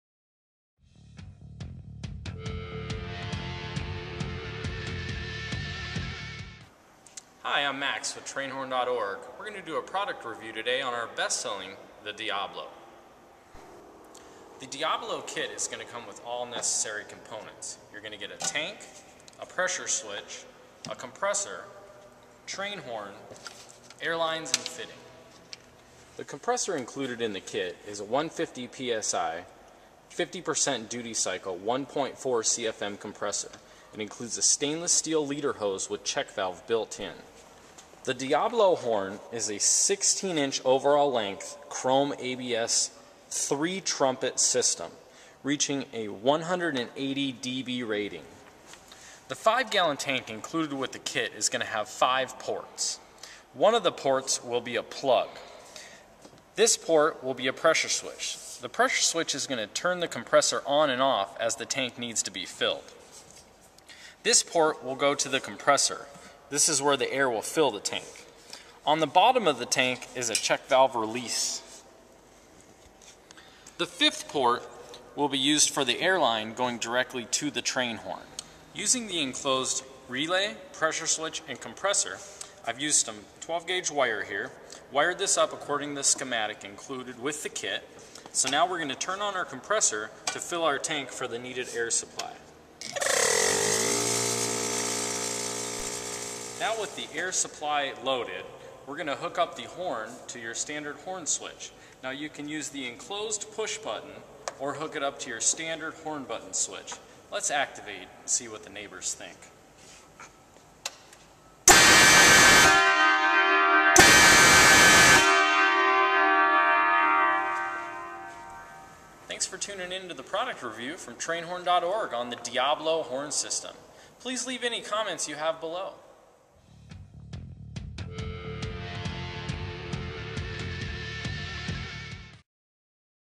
El Diablo Air Horn - Product Overview & Demonstration
Having trouble following the installation schematic included with your El Diablo air horn kit? If so, watch this video as our automotive guru Max explains the components included in the kit and demonstrates switch setup and proper wiring. The visuals in this review video should help make installation of the 150 psi compressor and the rest of the parts easy to follow. for all of your air horn needs.